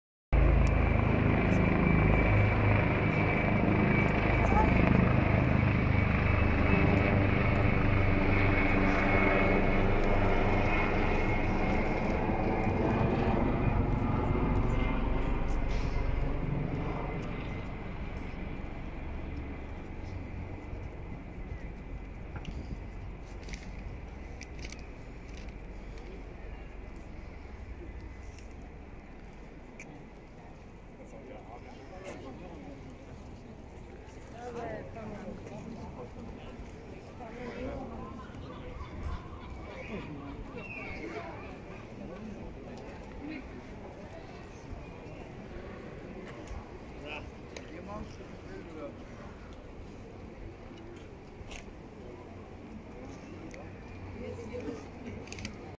A view into the Colosseum in Rome
Overview shot of the Colosseum in Roma. Walking into Ancient history 🤩